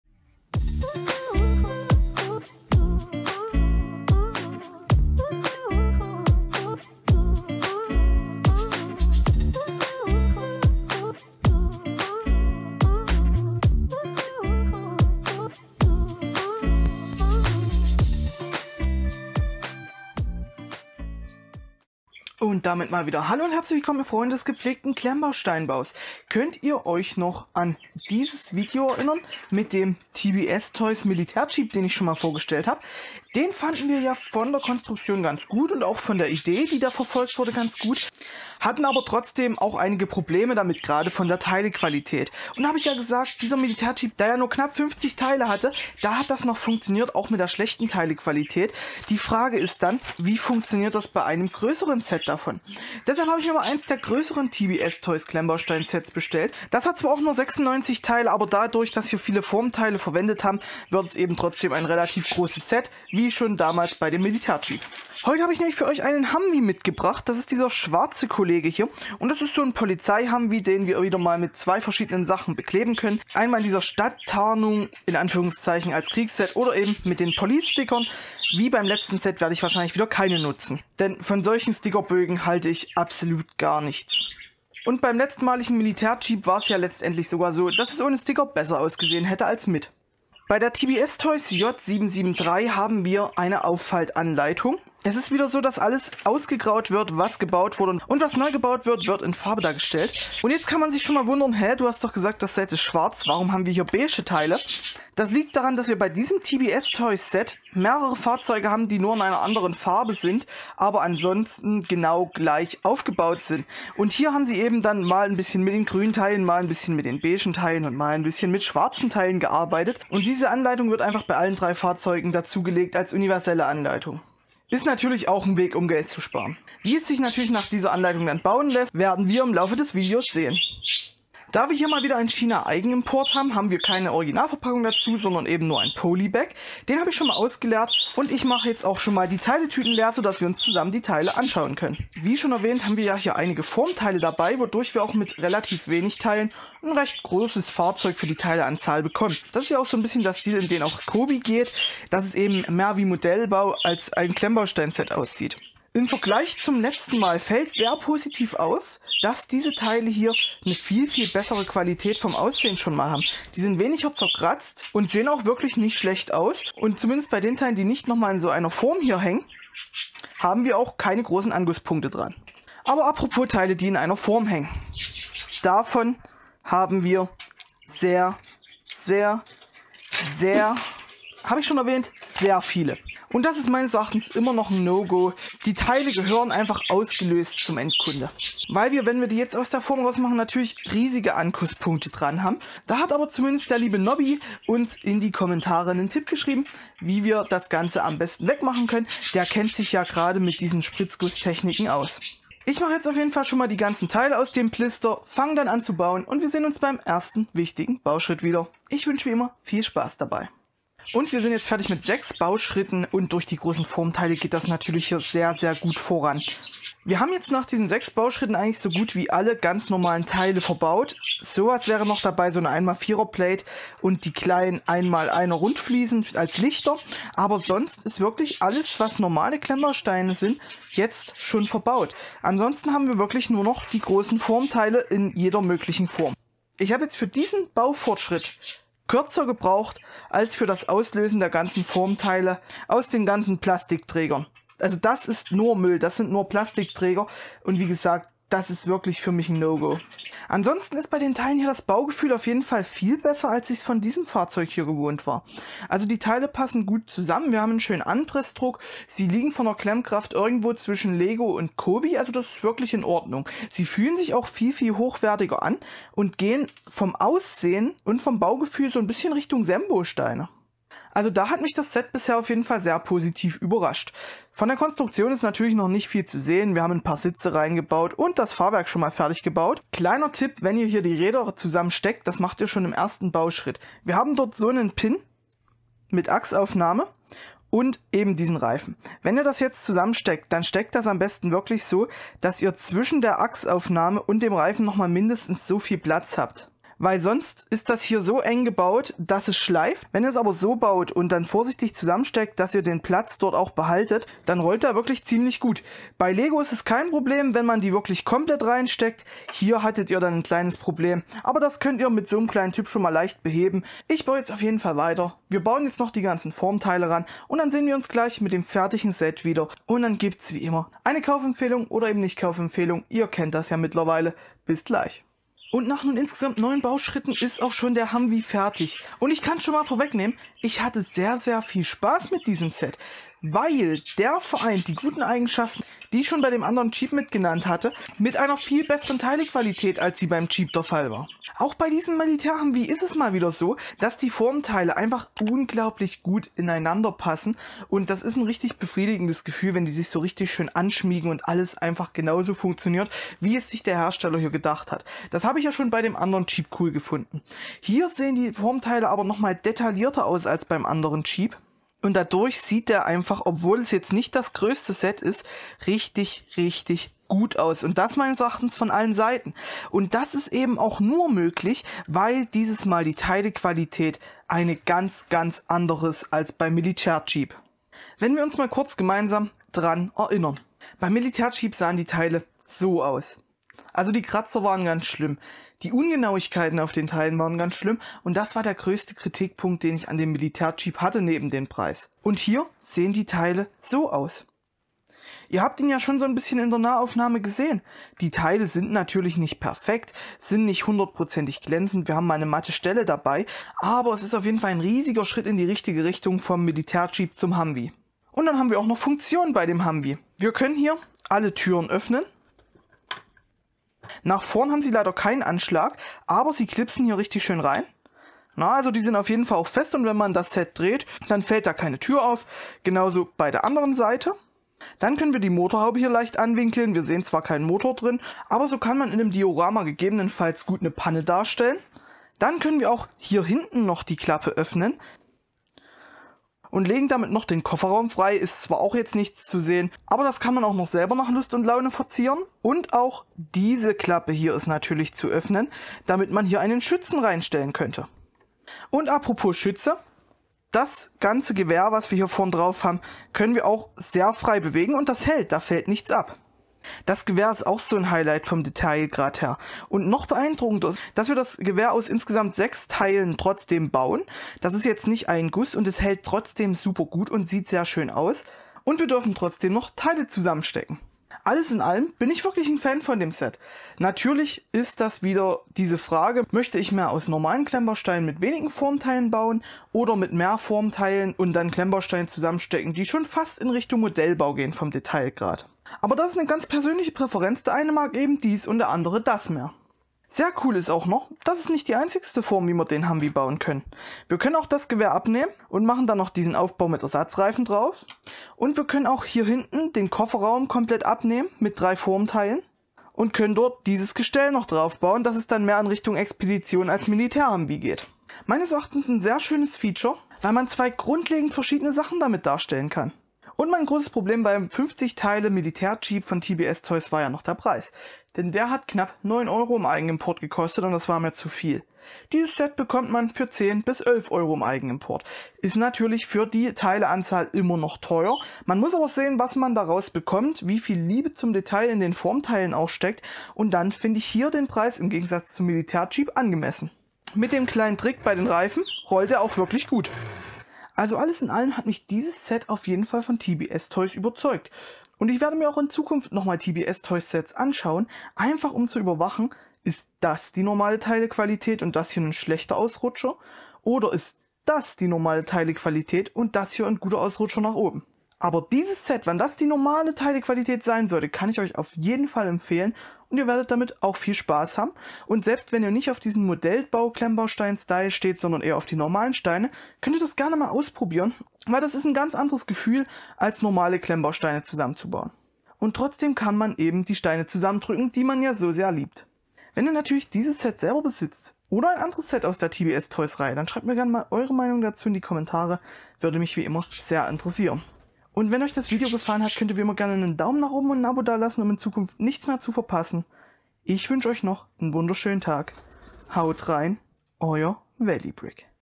Eine viel bessere TEILEQUALITÄT aber ein großes PROBLEM!! TBS TOYS J773 Klemmbaustein Humvee Review
Hallo liebe Freunde der gepflegten Klemmbaustein Baukunst. In diesem Video geht es um ein Set von TBS TOYS! Was kann der Klemmbaustein Humvee aus der Militär / Police Reihe? Welche Steinequalität erwartet uns?! Viel Spaß dabei :).

Gibt es da nicht  :D